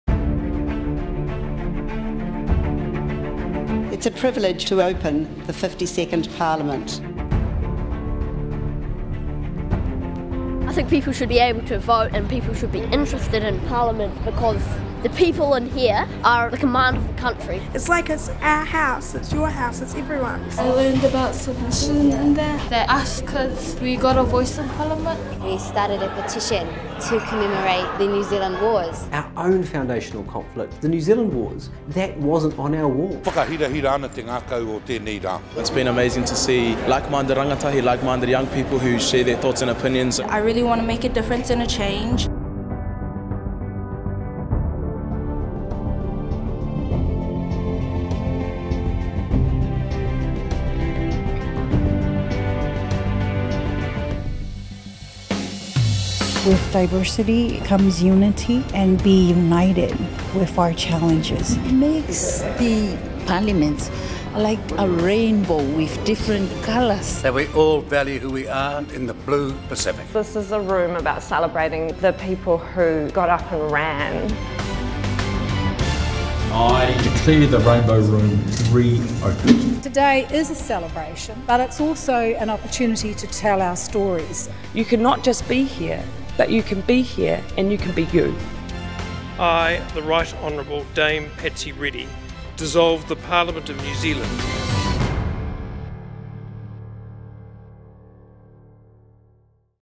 Highlights of 52nd Parliament | NZ Parliament
"It's our House, it's your House, it's everyone's."

From the reopening of the Rainbow Room to the Epidemic Response Committee meeting online, a lot has happened in the 52nd Parliament. Check out some of the ways people have engaged with Parliament in our highlights reel.

We can’t wait to see what the 53rd Parliament will bring!